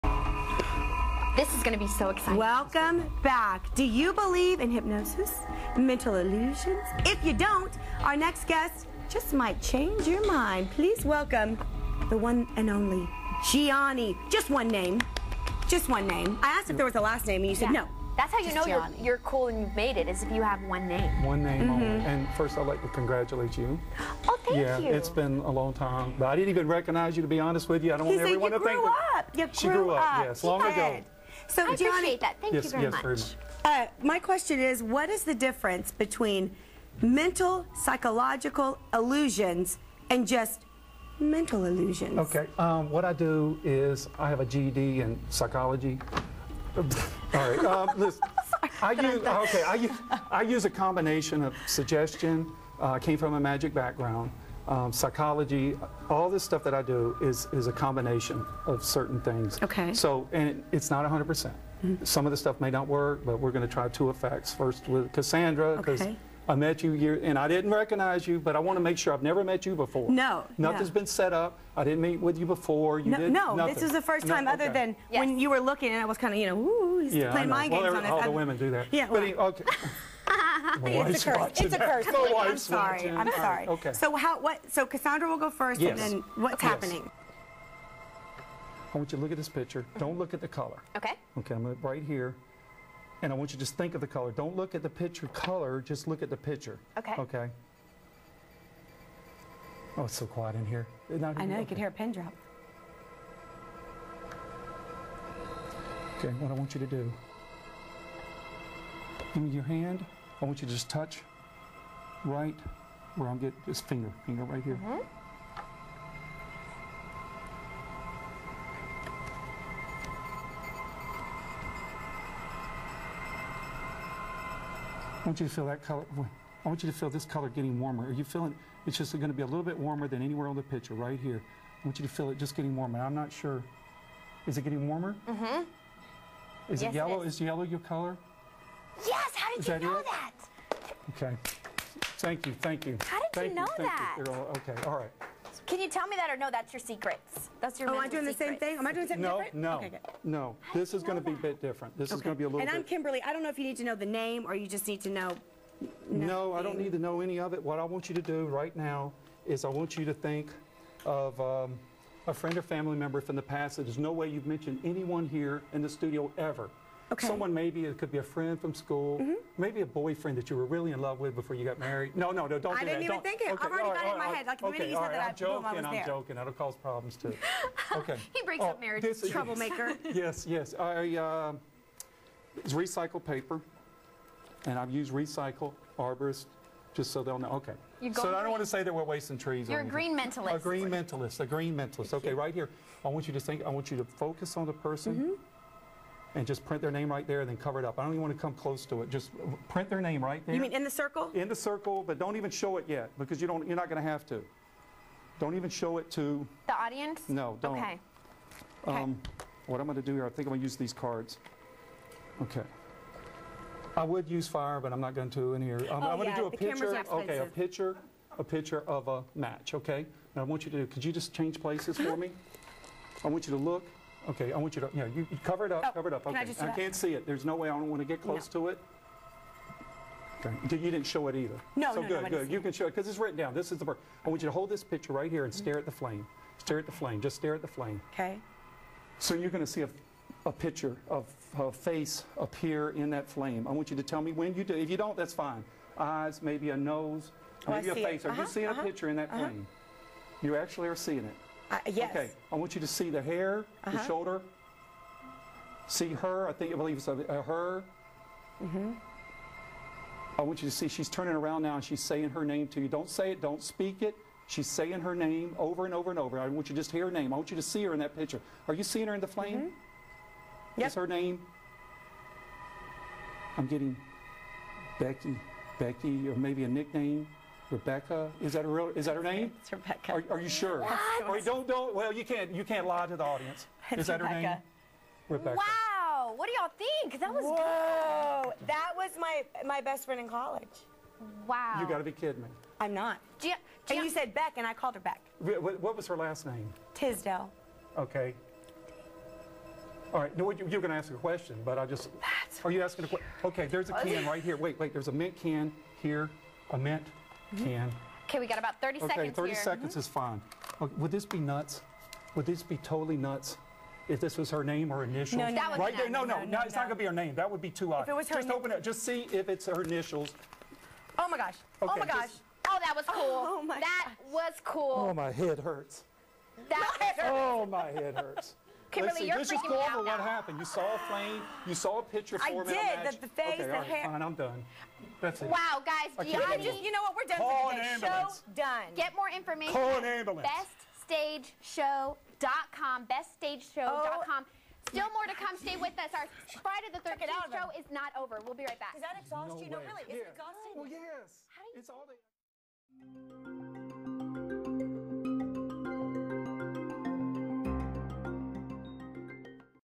Company Party Entertainment - San Antonio-Austin-Dallas
Giani as a guest on Fox's KABB-TV performing mindreading experiments.  Perfect party entertainment in Austin and Dallas.  Ask for specials for Dallas Conventions.